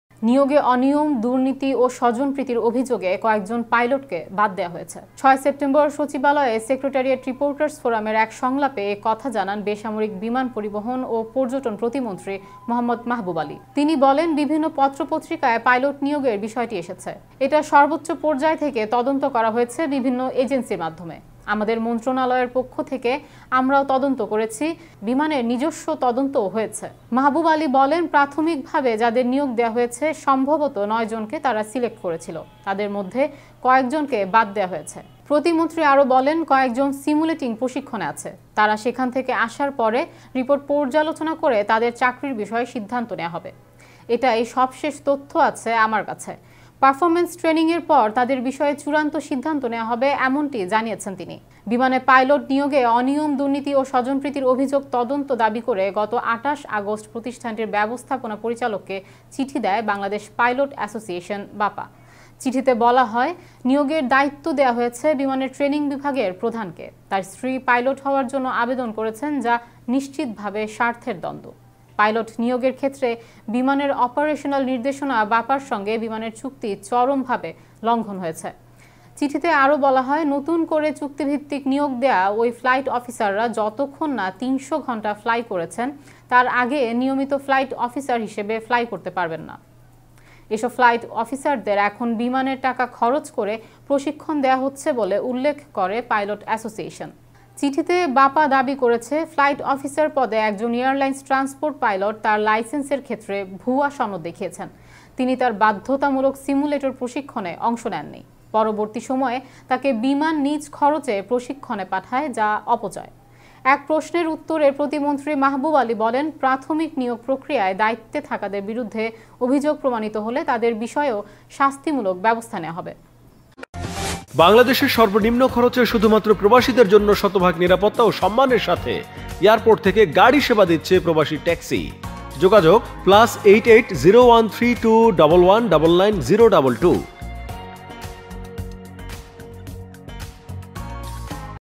দুর্নীতিগ্রস্ত পাইলটদের বিরুদ্ধে ব্যবস্থা নেবে বিমান | Biman | Flim Bazar
- Team Probash